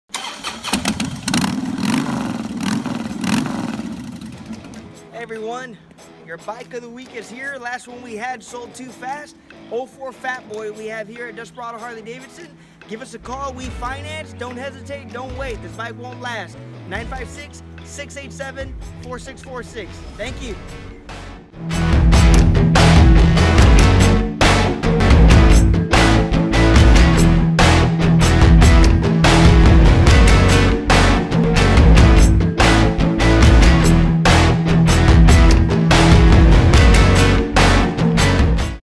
Desperado H-D: Bike of The Week - 2004 Softail Fat Boy
Emmanuel Sales Manager brings you this week's pre-owned Bike of The Week! 2004 Softail Fat Boy, carbureted and ready for the road, this Softail is packing custom exhaust and a backrest for your passenger. Don't wait, ride today!